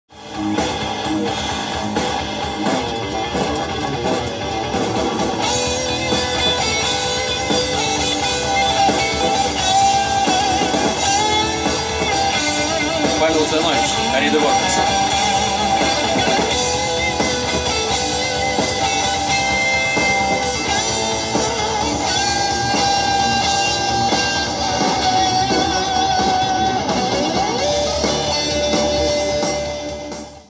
Arida Vortex - новый участник, гитарист Павел Жданович!
Маленький новогодний подарок ) У нас новый гитарист!